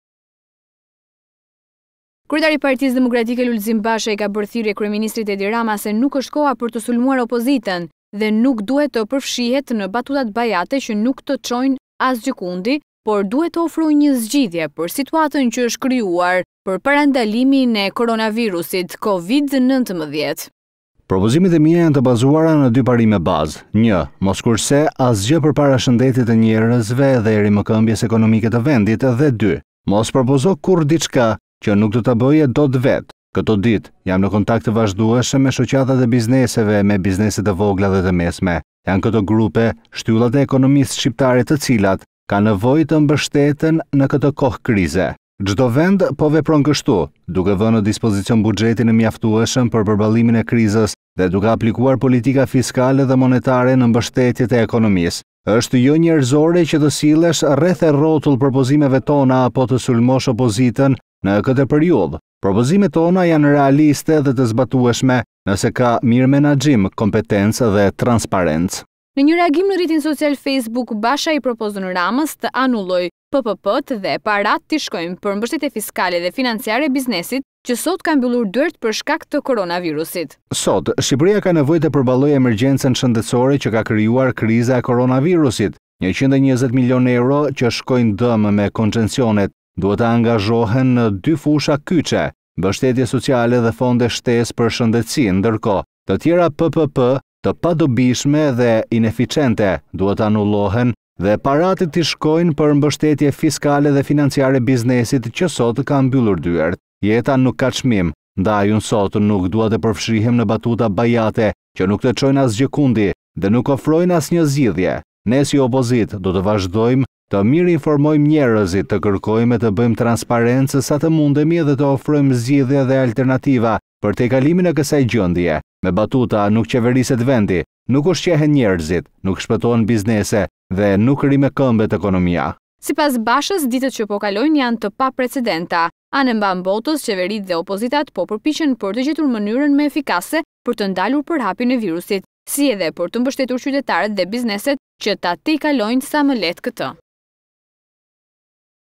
Mos propozo diçka qe vete s’do ta beje. Basha thirrje Rames: Leri batutat bajate, anulo PPP...
Mos propozo diçka qe vete s’do ta beje. Basha thirrje Rames: Leri batutat bajate, anulo PPP, mbeshtetje fiskale per biznesin.